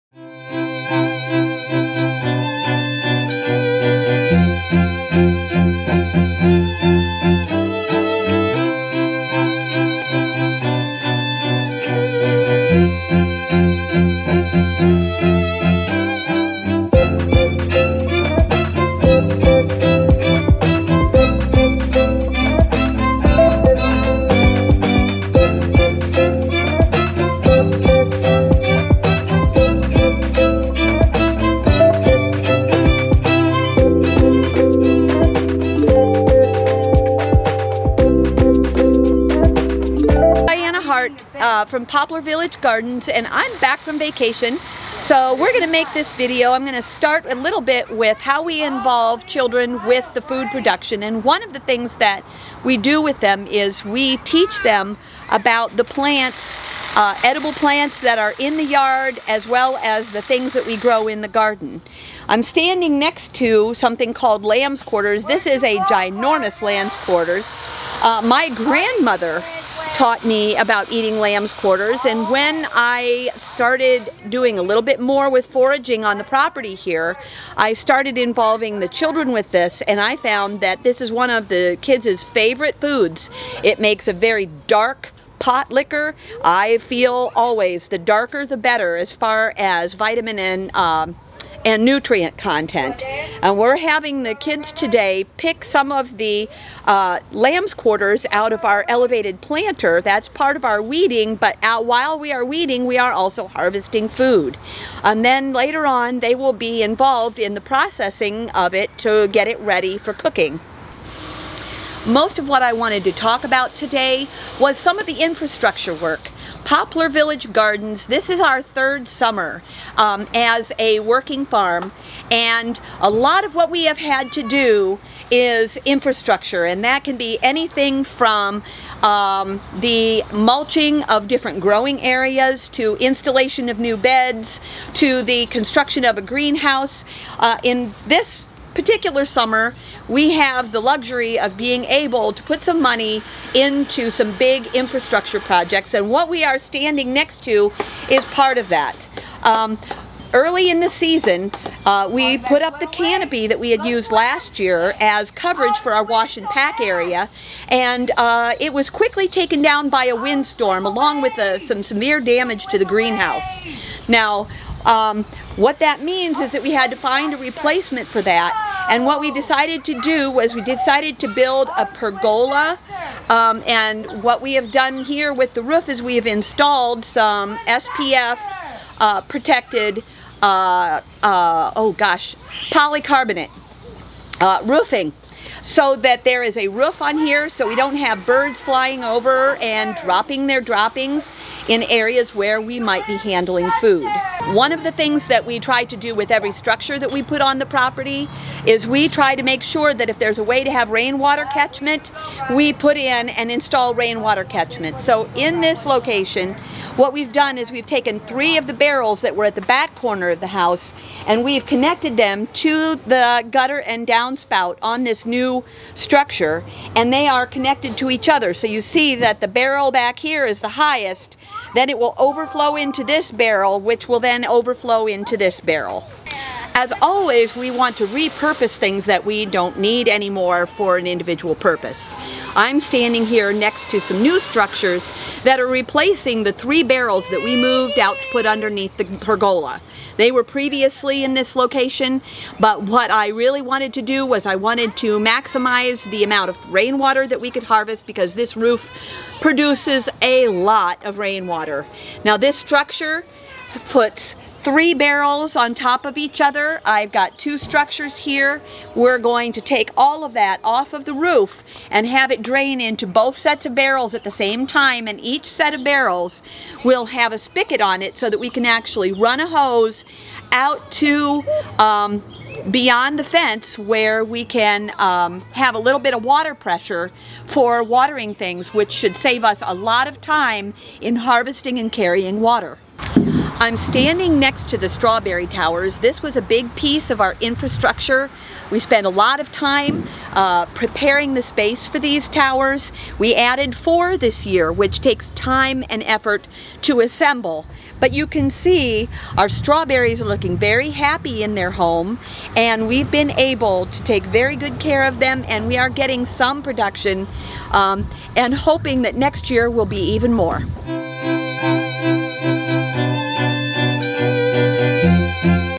Diana discusses the children's participation in the garden the infrastructure for this season.
New rainwater collection expands our capacity to harvest rain.  Children prepare greens for lunch.